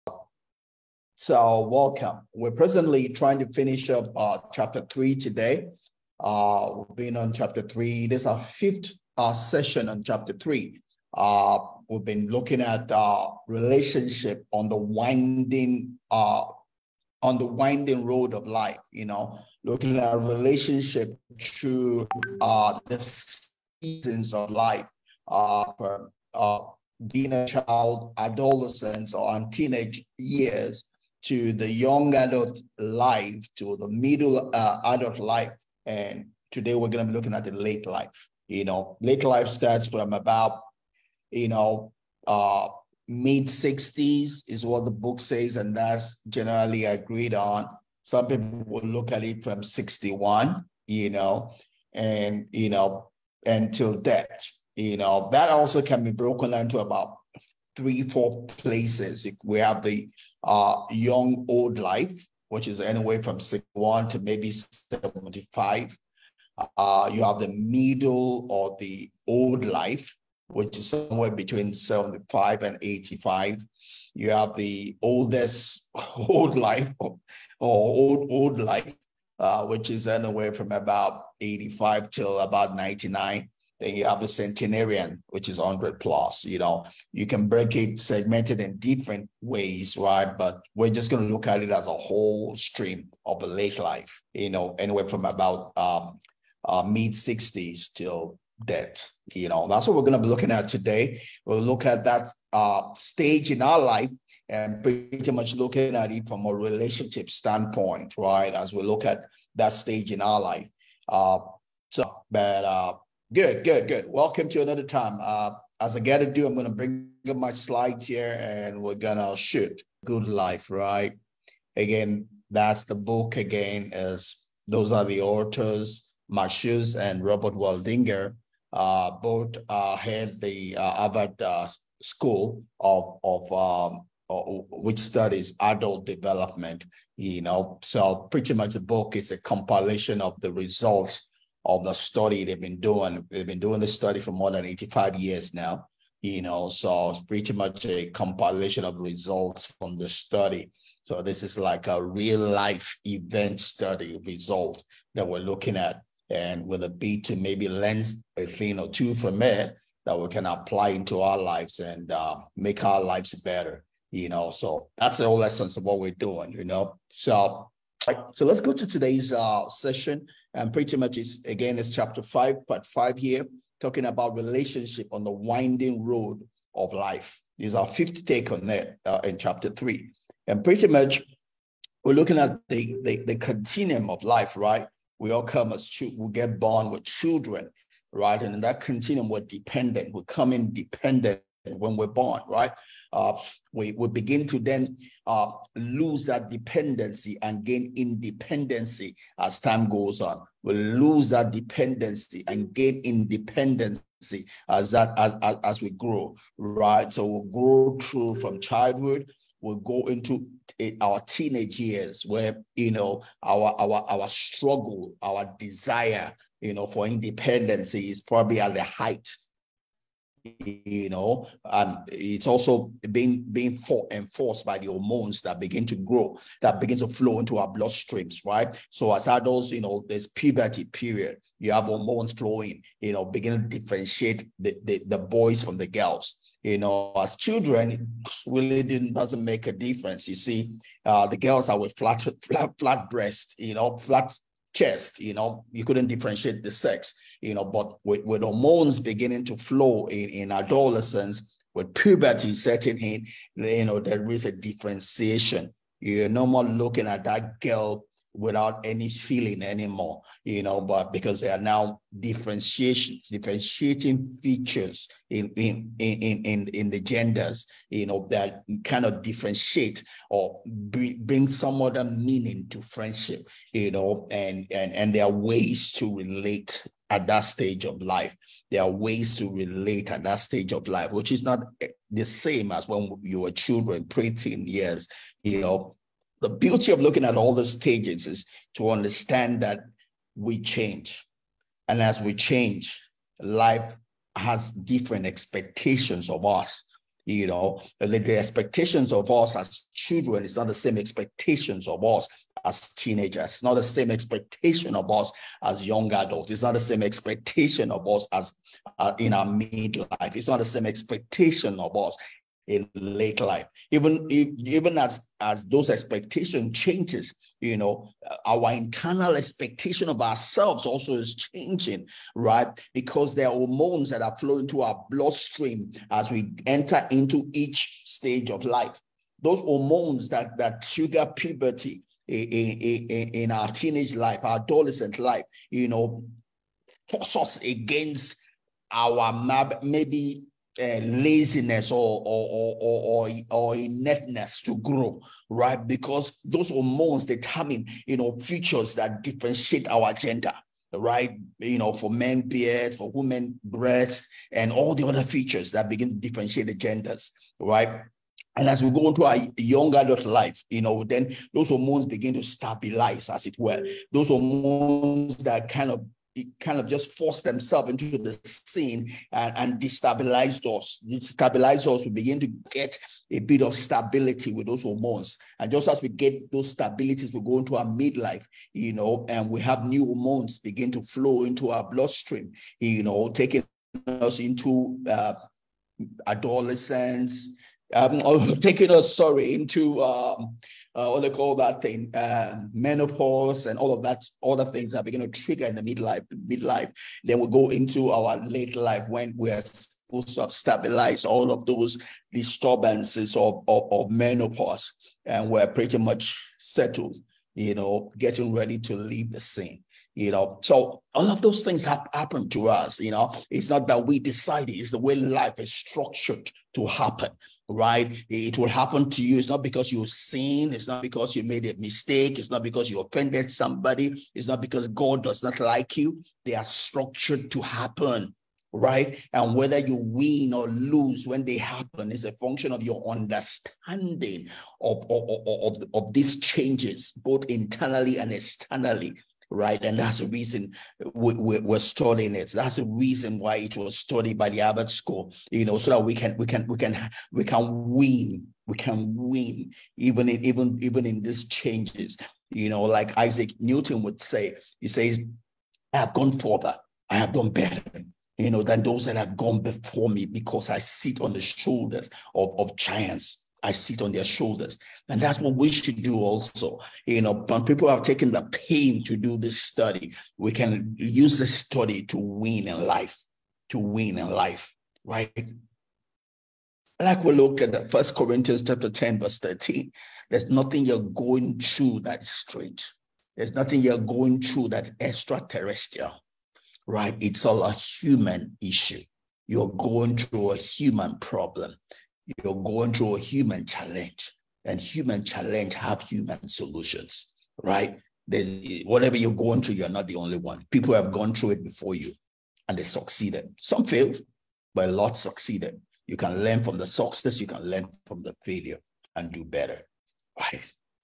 Happy | Part Thirty-Three | Late life | Part One | The Saint.
This is an extract from our weekly weekend Book Club.

We are creatures of belief. That is the way we captain our subconsciousness to function and hence master our destiny.

Series: Book Club: Happy.